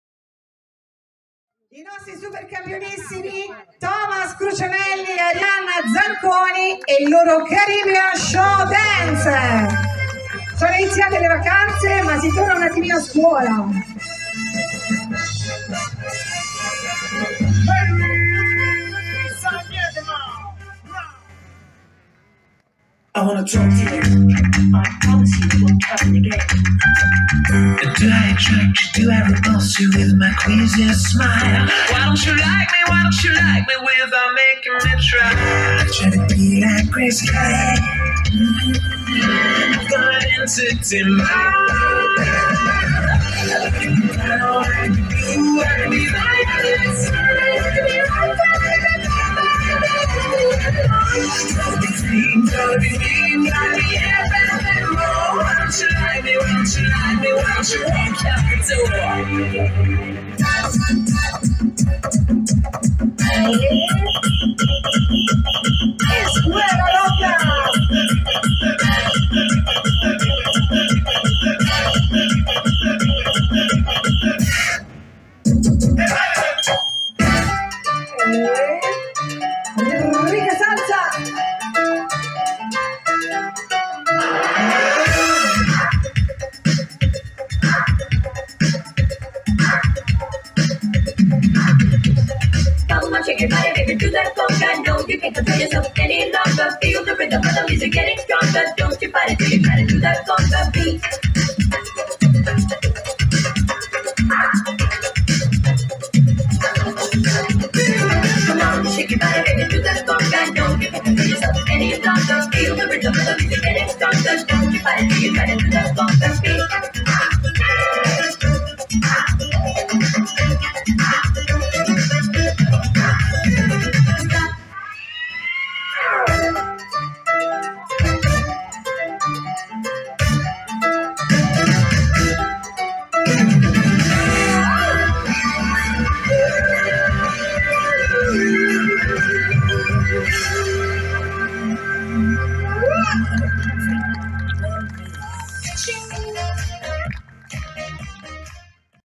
dom 7 ago 2022 Fashion Tour caribbean show dance La scuola - Thomas Crucianelli ed Arianna Zanconi
23a Sagra Pizza e Birra -
Centro Ricreativo Associativo Zona Industriale Corridonia